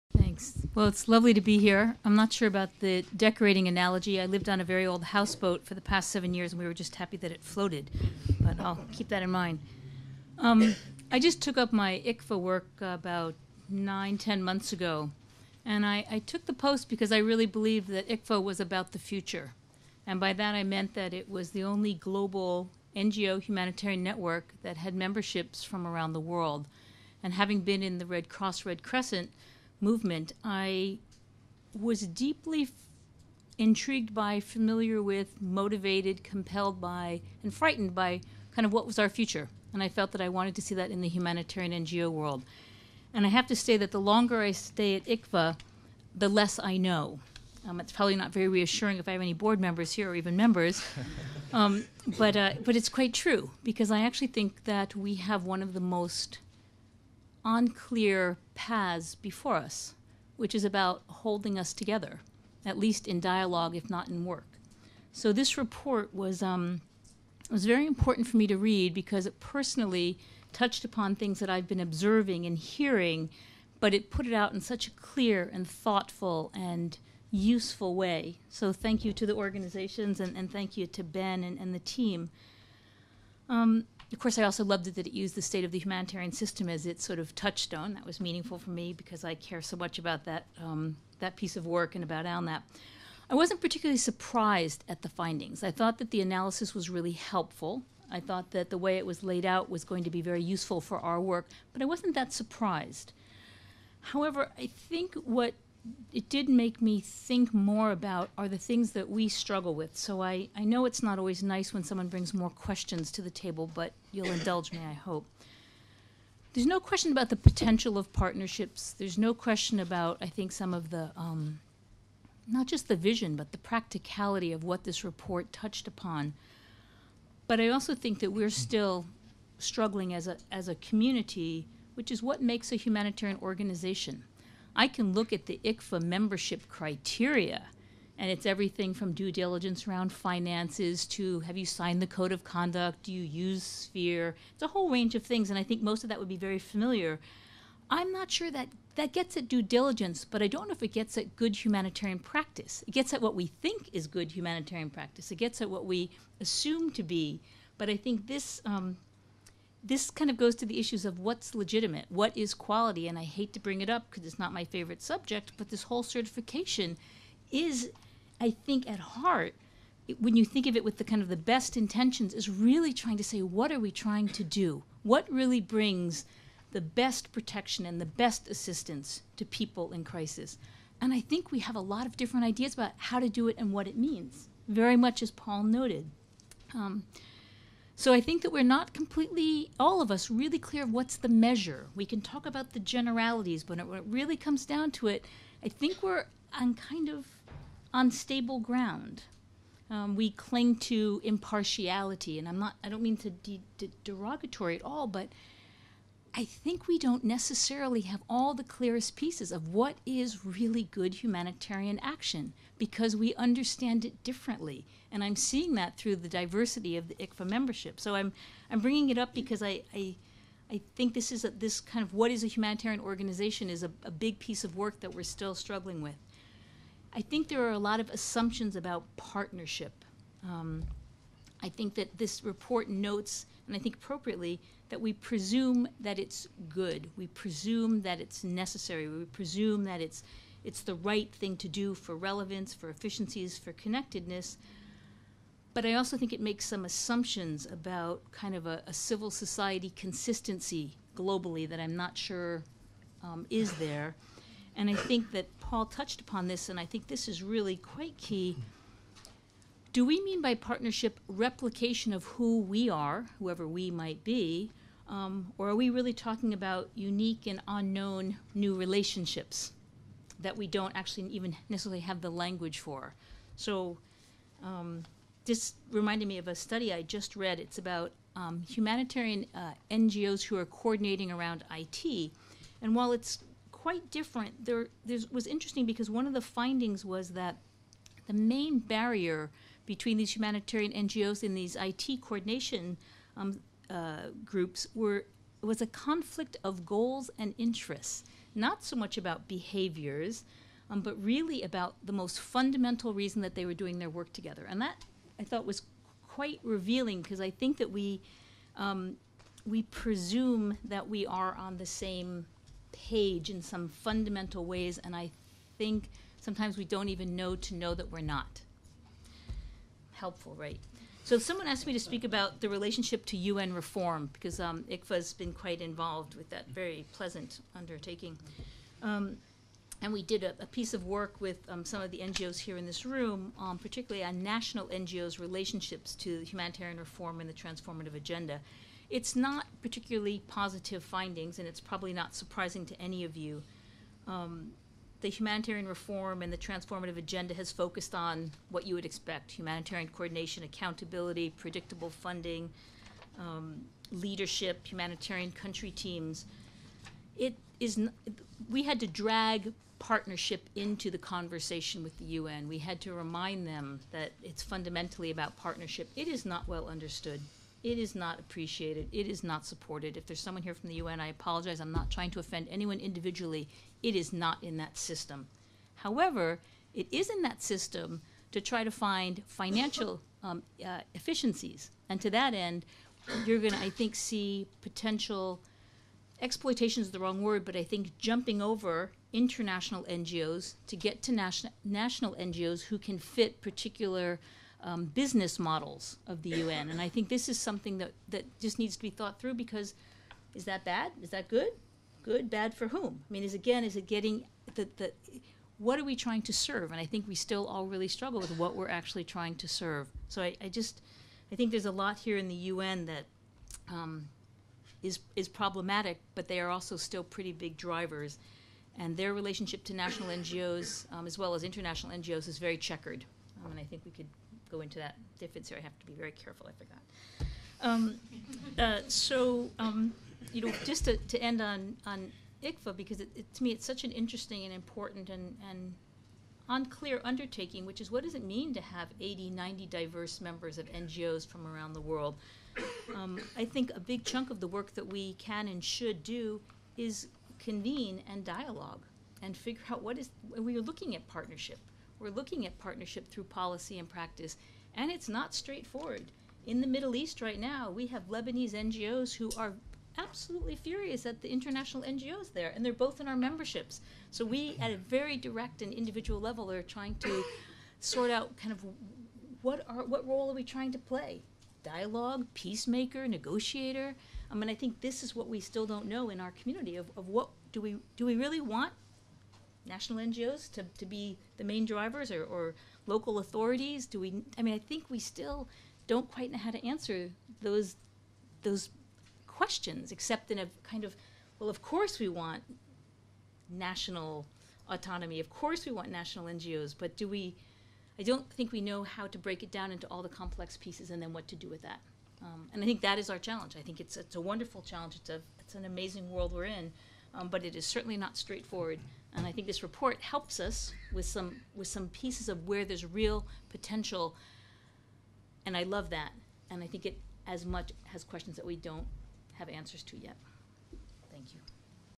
Nan Buzard - Partnering in emergencies
Nan Buzard - Executive Director, ICVA - responds to findings from the report 'Missed Opportunities: the case for strengthening local partnership-based humanitarian responses'. This event - Partnering in emergencies - was held on 4 November at ODI offices, London.